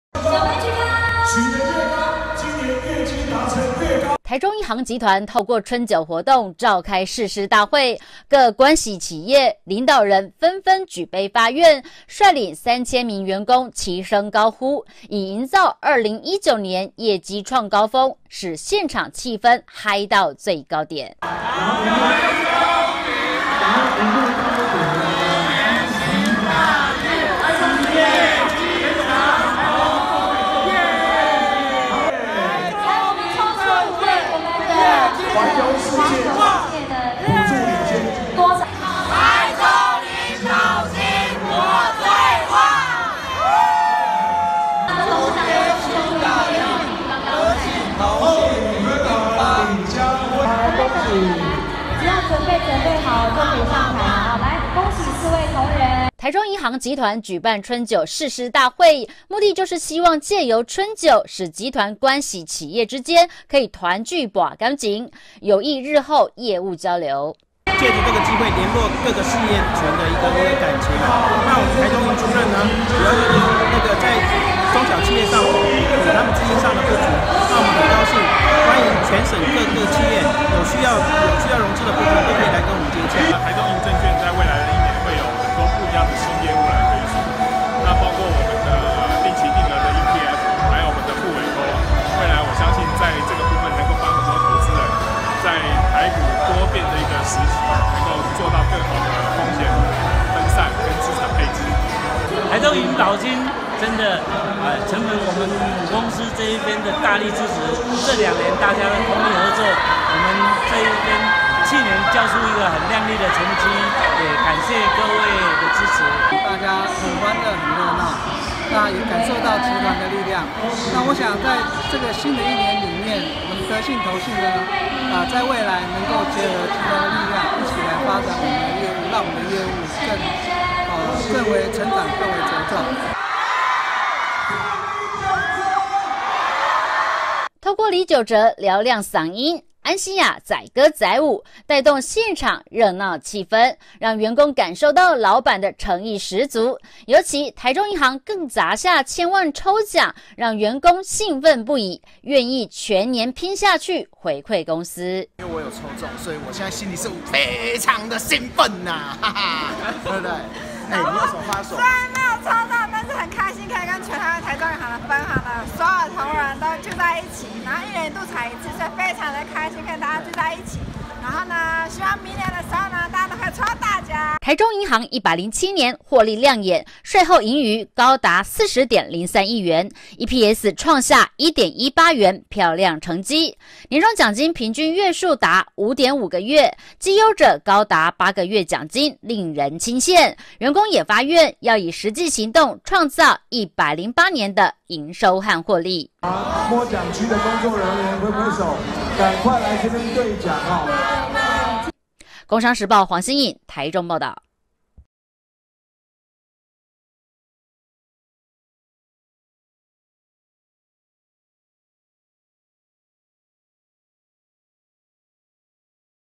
台中銀春酒凝共識 創新求變創高峰
讓您掌握先機，投資無往不利

成立滿65周年的台中銀行，日前於大台中國際會展中心舉辦集團春酒餐會，三千人齊聚一堂慶開春。